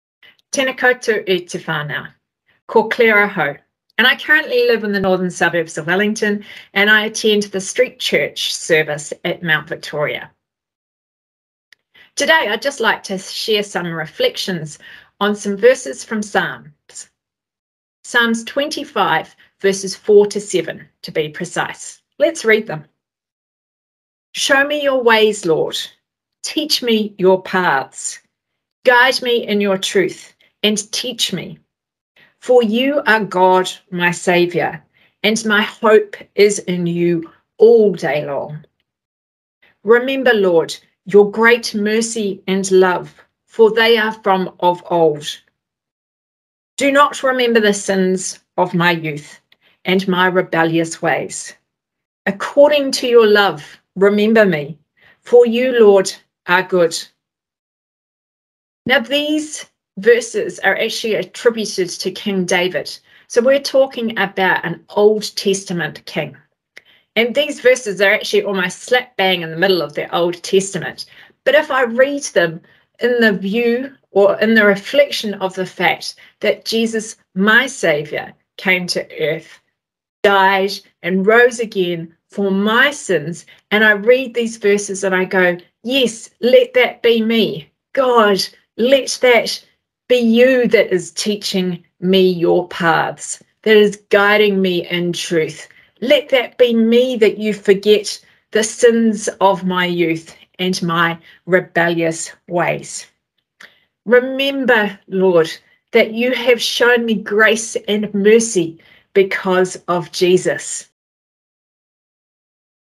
Claire's Story Of Hope
Claire shares her thoughts on hope. Could you use more hope in your life? Why not come to church this Christmas?